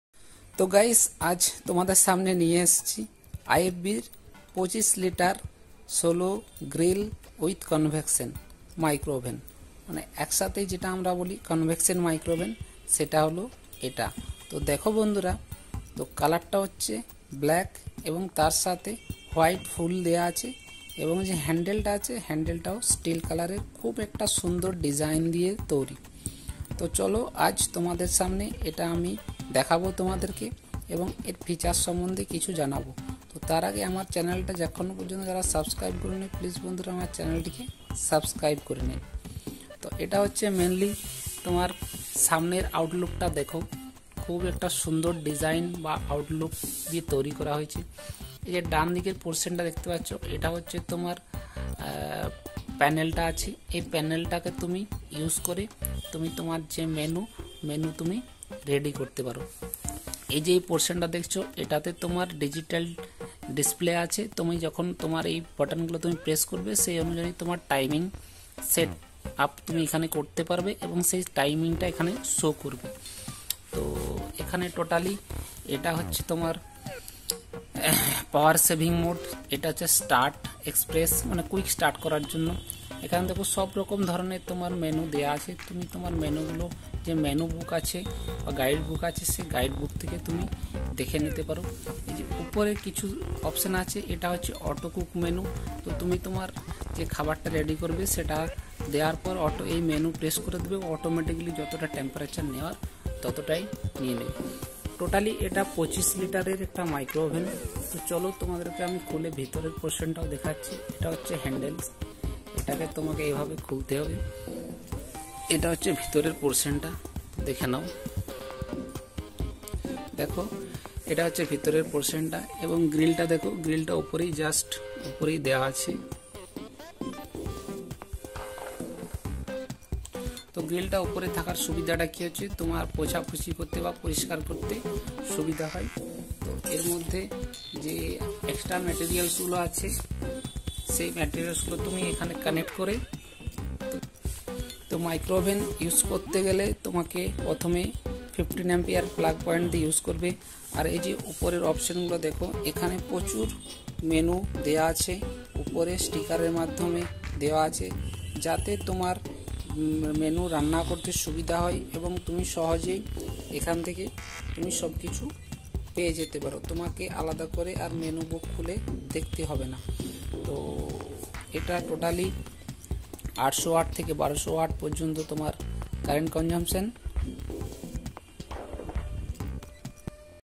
IFB 25 L Convection Microwave Oven (25BC4, Black, Floral Design, With Starter Kit)
Technical Details
Brand ‎IFB
Model ‎25BC4
Capacity ‎25 litres
Volume Capacity Name ‎25 litres
Part Number ‎25BC4
Special Features ‎Auto_cook
Oven Cooking Mode ‎Convection
Colour ‎Black
Defrost System ‎Defrost
Door Orientation ‎Left
Included Components ‎1 Microwave Oven, Warranty Card, User Manual and Starter Kit
Manufacturer ‎Guangdong Galanz Microwave Oven and Electrical Appliances Manufacturing Co. Ltd